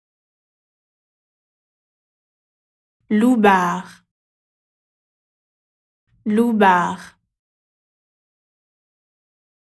How to pronounce LOUBARD in French
This video shows you how to pronounce LOUBARD in French. Speaker has an accent from Calais (Pas-de-Calais, Hauts-de-France). Collins Dictionary, the home of living language and pioneers of dictionary publishing